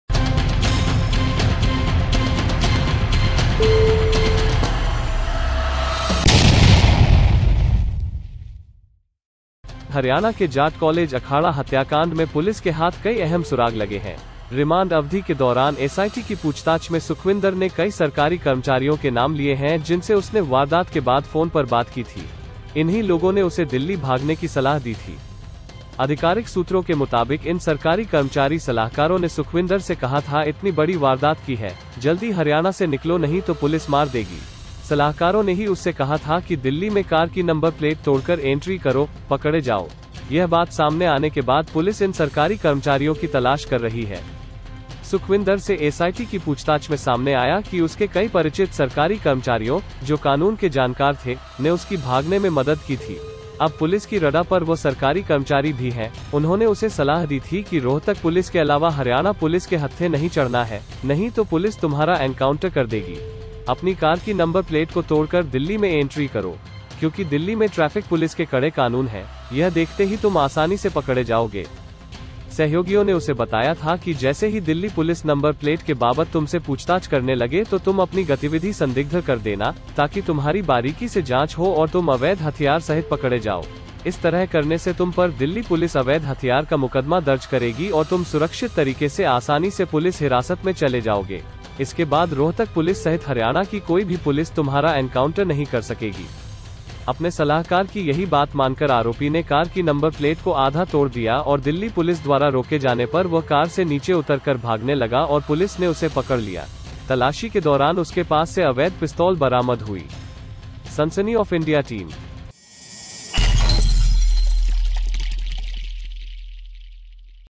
सुखविंदर ने खोला मुंह, अब फंसेंगे कई सरकारी अधिकारी !
हरियाणा के जाट कॉलेज अखाड़ा हत्याकांड में पुलिस के हाथ कई अहम सुराग लगे हैं। रिमांड अवधि के दौरान एसआईटी की पूछताछ में सुखविंदर ने कई सरकारी कर्मचारियों के नाम लिए हैं जिनसे उसने वारदात के बाद फोन पर बात की थी। इन्हीं लोगों ने उसे दिल्ली भागने की सलाह दी थी।

अधिकारिक सूत्रों के मुताबिक इन सरकारी कर्मचारी सलाहकारों ने सुखविंदर से कहा था इतनी बड़ी वारदात की है, जल्दी हरियाणा से निकलो नहीं तो पुलिस मार देगी। सलाहकारों ने ही उससे कहा था कि दिल्ली में कार की नंबर प्लेट तोड़कर एंट्री करो, पकड़े जाओ। यह बात सामने आने के बाद पुलिस इन सरकारी कर्मचारियों की तलाश कर रही है।

सुखविंदर से एसआईटी की पूछताछ में सामने आया कि उसके कई परिचित सरकारी कर्मचारियों, जो कानून के जानकार थे, ने उसकी भागने में मदद की थी। अब पुलिस की रडार पर वह सरकारी कर्मचारी भी हैं, उन्होंने उसे सलाह दी थी कि रोहतक पुलिस के अलावा हरियाणा पुलिस के हत्थे नहीं चढ़ना है, नहीं तो पुलिस तुम्हारा एनकाउंटर कर देगी। अपनी कार की नंबर प्लेट को तोड़कर दिल्ली में एंट्री करो। क्योंकि दिल्ली में ट्रैफिक पुलिस के कड़े कानून हैं, यह देखते ही तुम आसानी से पकड़े जाओगे।

सहयोगियों ने उसे बताया था कि जैसे ही दिल्ली पुलिस नंबर प्लेट के बाबत तुमसे पूछताछ करने लगे तो तुम अपनी गतिविधि संदिग्ध कर देना, ताकि तुम्हारी बारीकी से जांच हो और तुम अवैध हथियार सहित पकड़े जाओ। इस तरह करने से तुम पर दिल्ली पुलिस अवैध हथियार का मुकदमा दर्ज करेगी और तुम सुरक्षित तरीके से आसानी से पुलिस हिरासत में चले जाओगे। इसके बाद रोहतक पुलिस सहित हरियाणा की कोई भी पुलिस तुम्हारा एनकाउंटर नहीं कर सकेगी।

अपने सलाहकार की यही बात मानकर आरोपी ने कार की नंबर प्लेट को आधा तोड़ दिया और दिल्ली पुलिस द्वारा रोके जाने पर वह कार से नीचे उतर कर भागने लगा और पुलिस ने उसे पकड़ लिया। तलाशी के दौरान उसके पास से अवैध पिस्तौल बरामद हुई।

सनसनी ऑफ़ इंडिया टीम